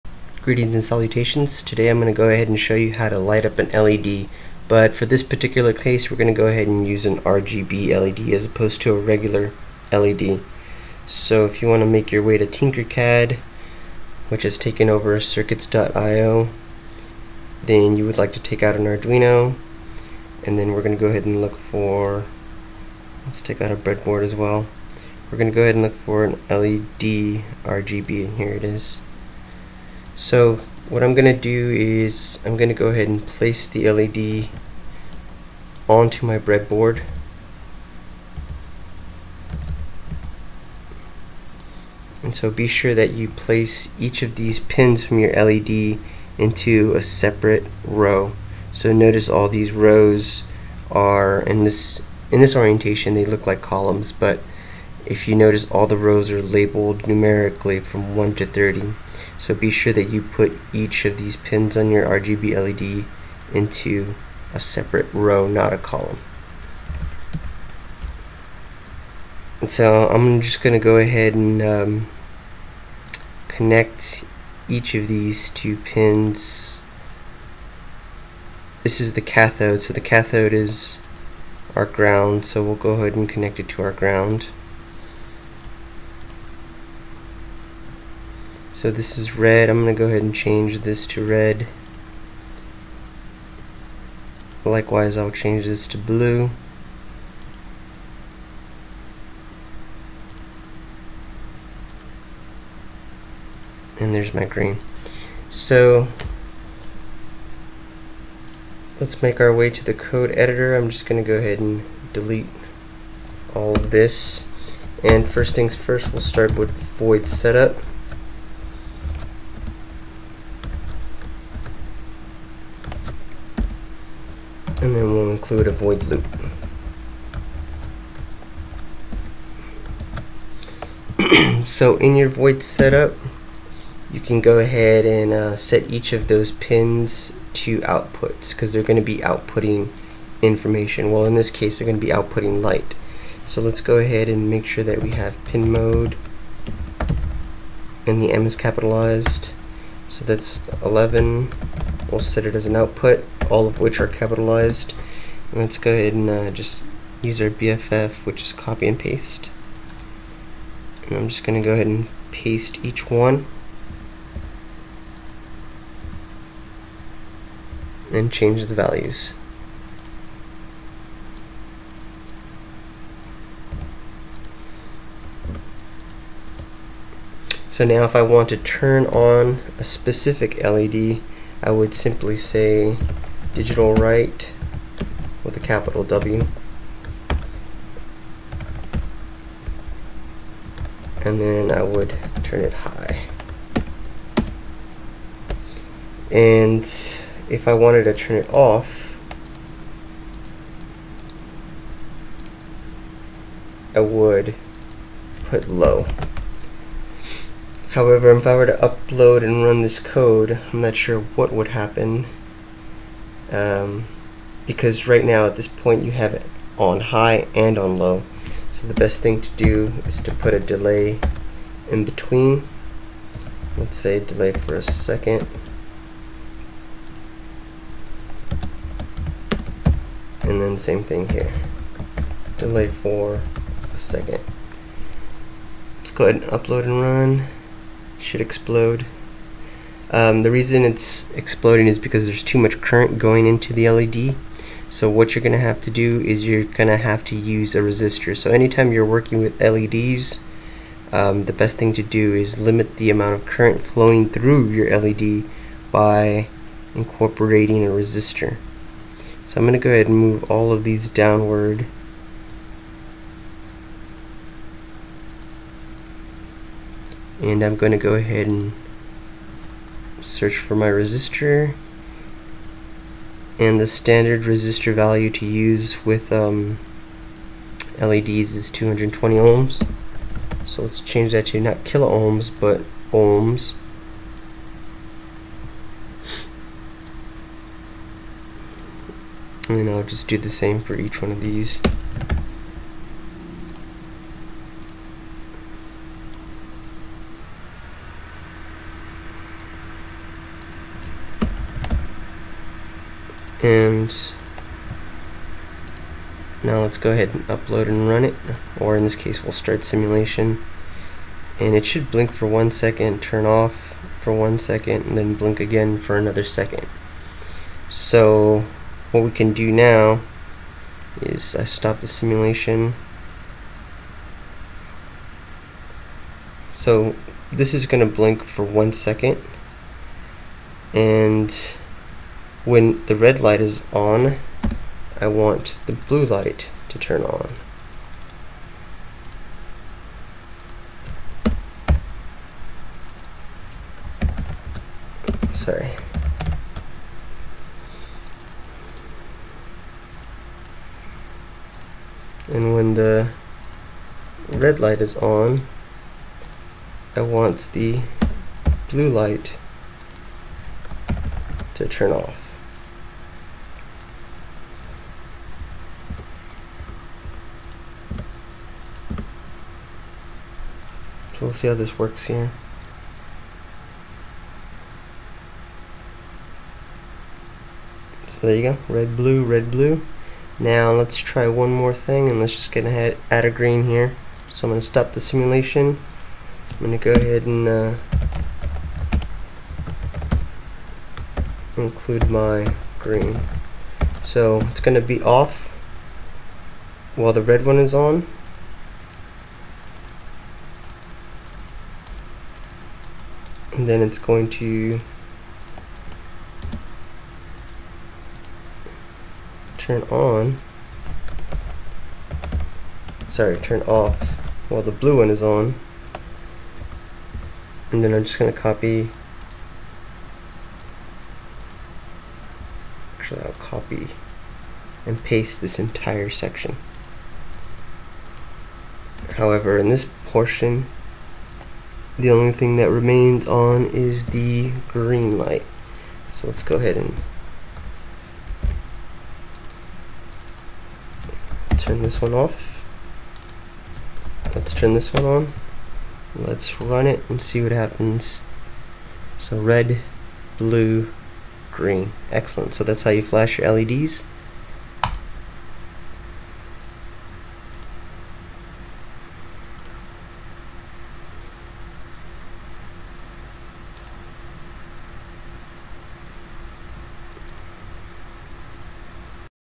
How to set up an RGB on Tinkercad
This tutorial will show you how to blink an RGB LED